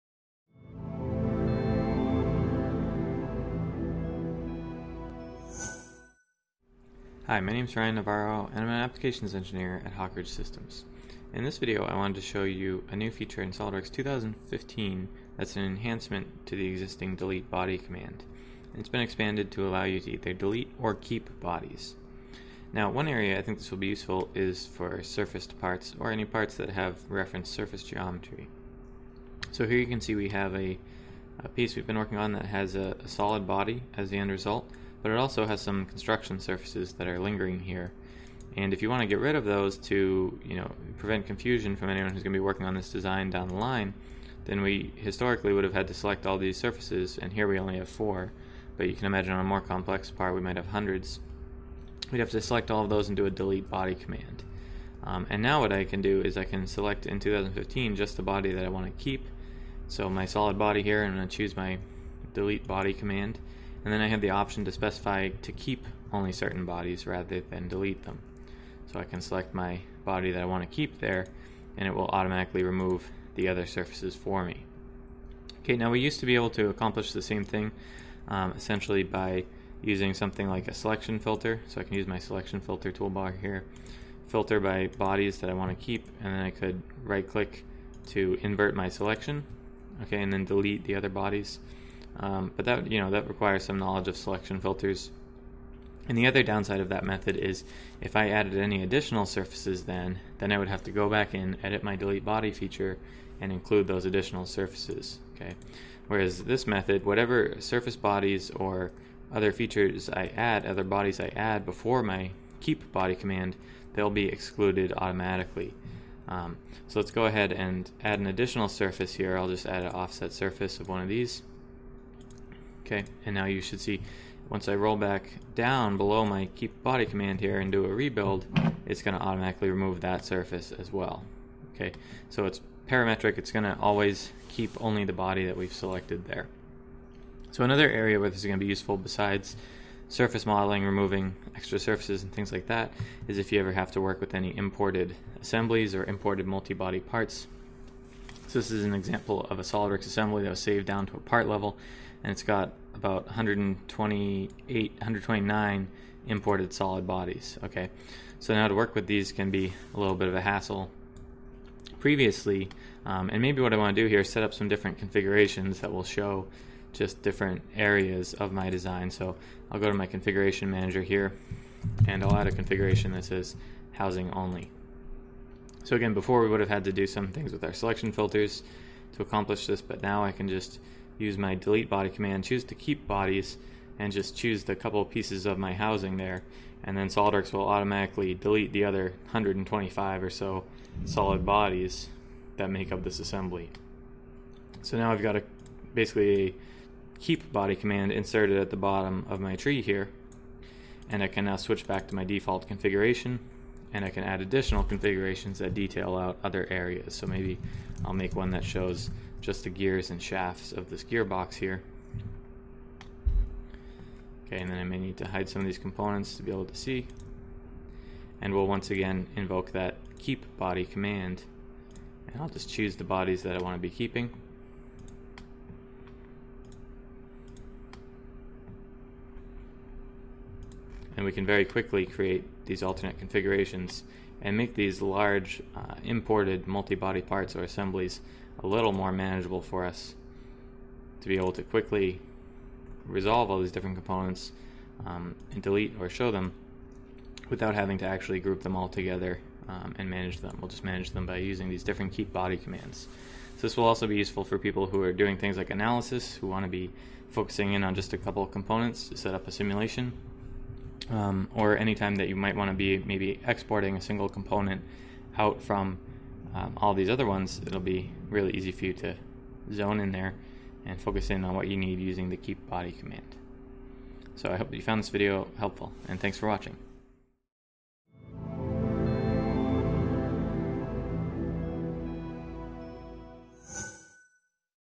SOLIDWORKS - New in 2015: Delete or Keep Body Command
Delete body command has been expanded to include "keep bodies" option, which is very useful when you want to remove all but a few bodies.